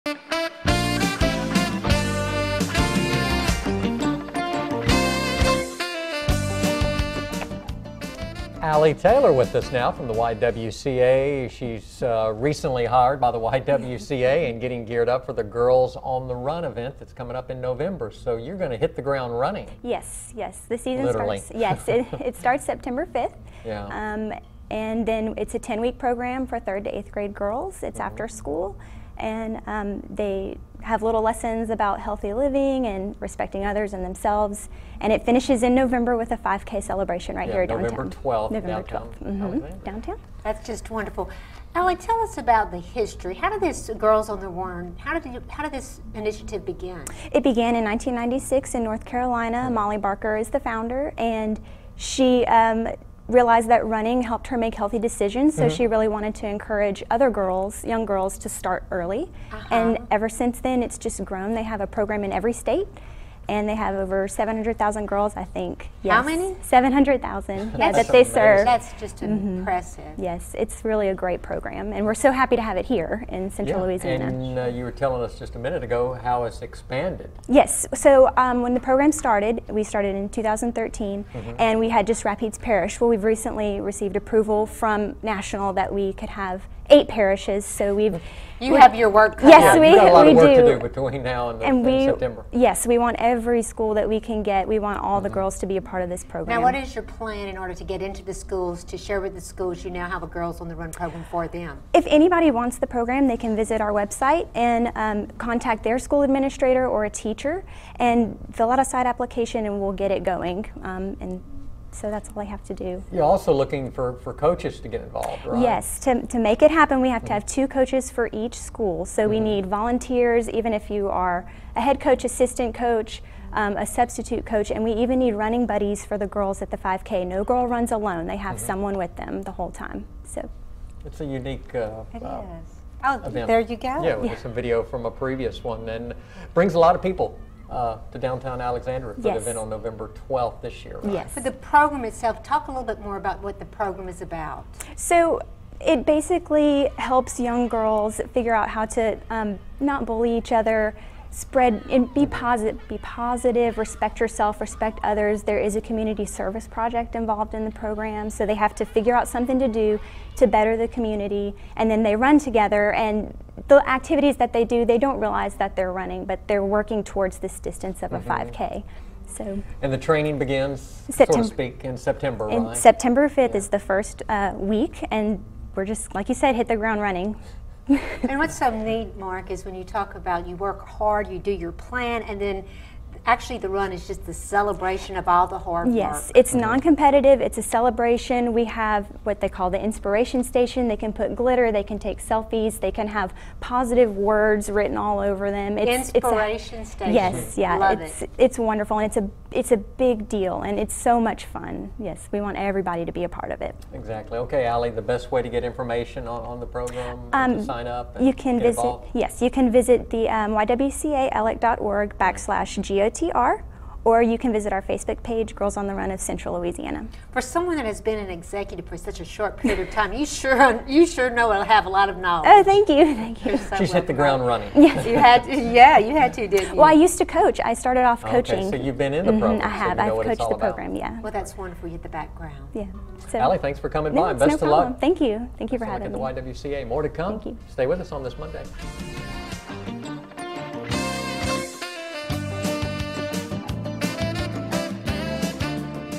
Girls on the Run Preview
Ali Taylor talks about the upcoming Girls on the Run happening in November